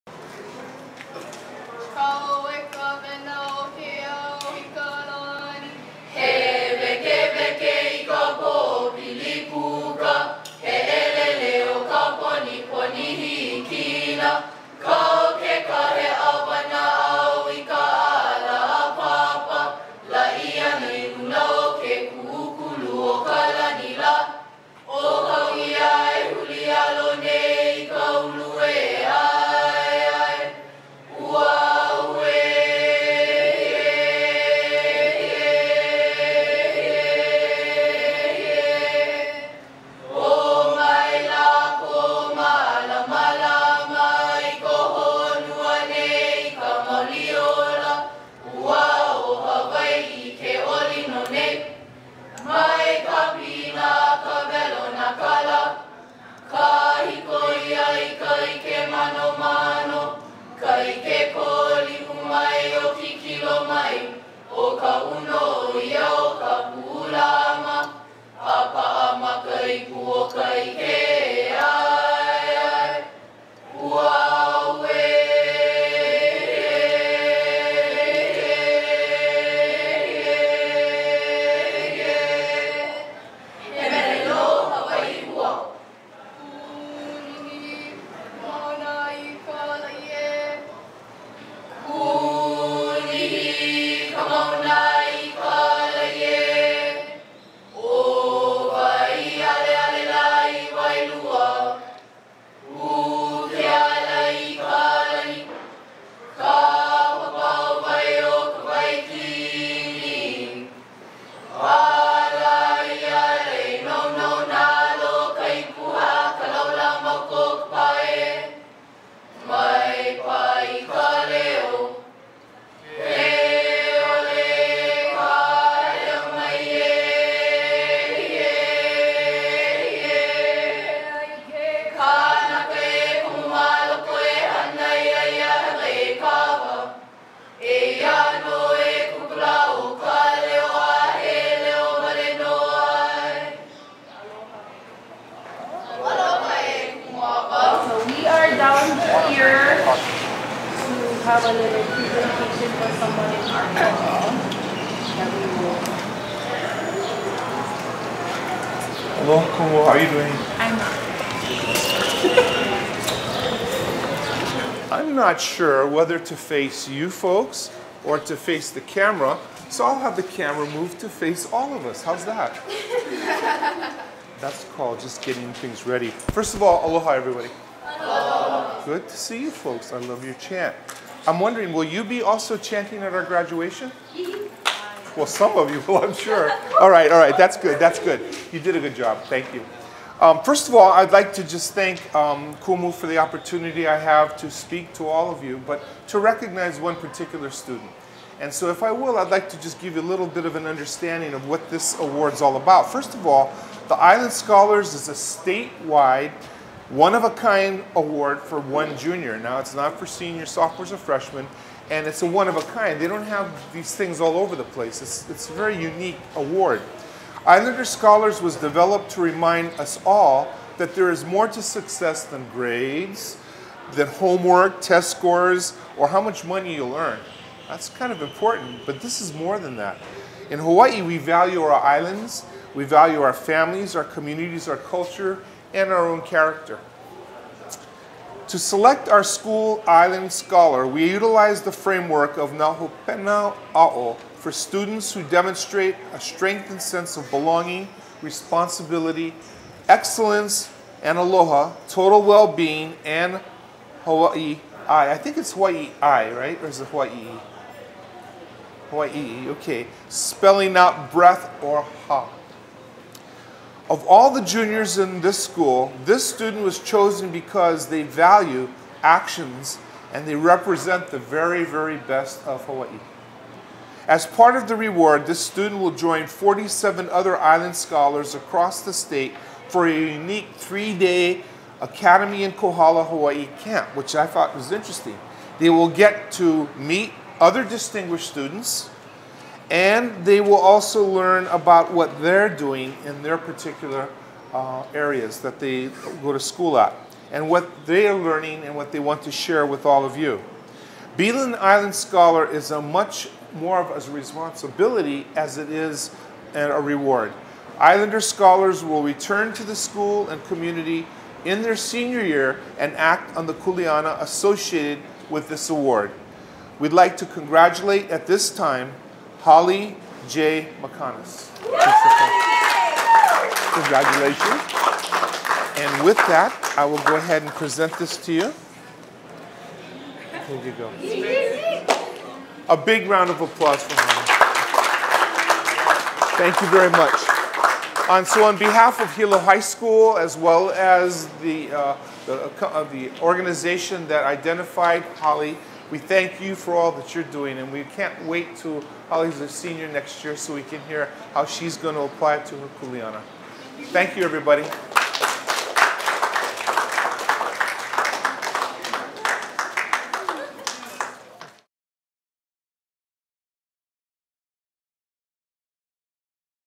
03/28/2019 Islander Scholar Presentation
Principal Robert Dircks selects the student representing Hilo High School for the Islander Institute Scholars Program.

Every year, public school principals at traditional and charter high schools statewide are invited to select one junior in their school to be an Islander Scholar based on six comprehensive learning outcomes rooted in Hawaiʻi’s culture and environment.

Hilo High School
556 Waianuenue Avenue
Hilo, HI 96720